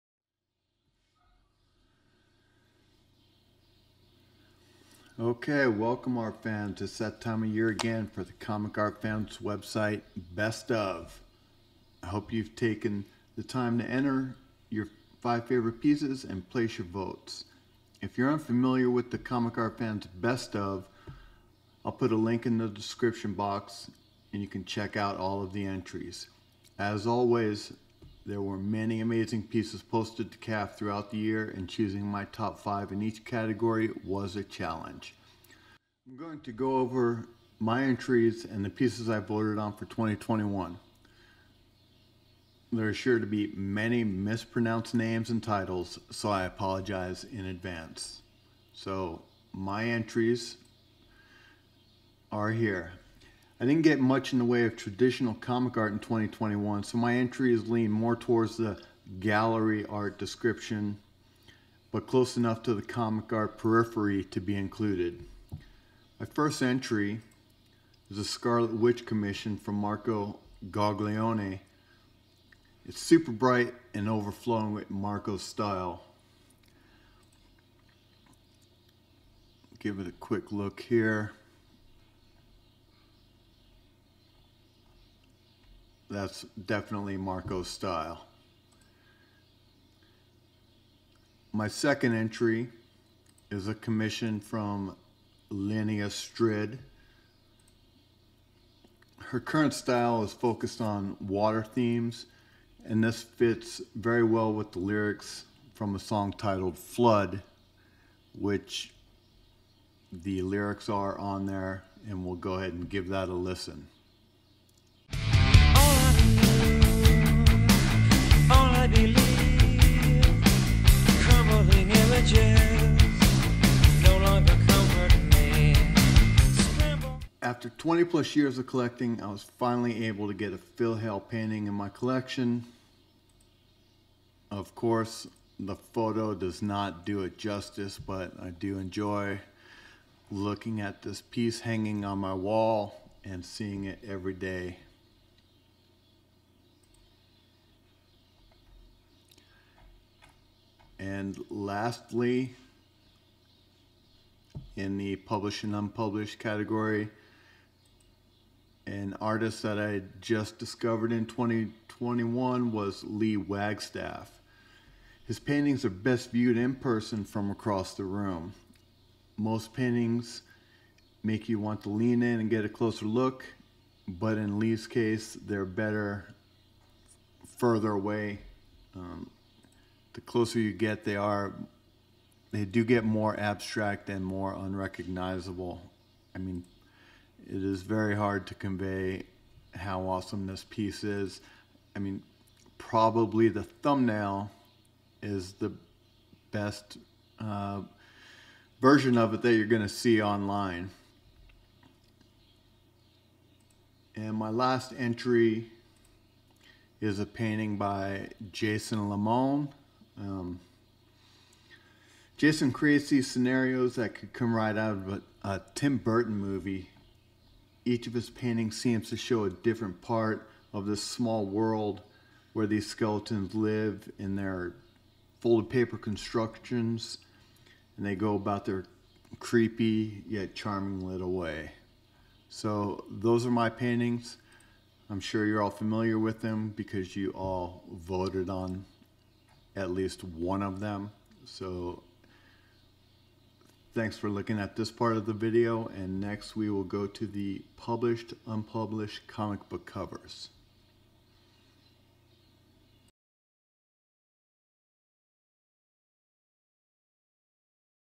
ComicArtFans Best Of 2021 - MY ENTRIES | NSNart
Each year the website ComicArtFans.com has a vote for the best pieces added the previous year.
These are the five pieces I chose to enter.   Includes art from:
 - Marco Gauglione
 - Linnea Strid
 - Phil Hale
 - Lee Wagstaff
 - Jason Limon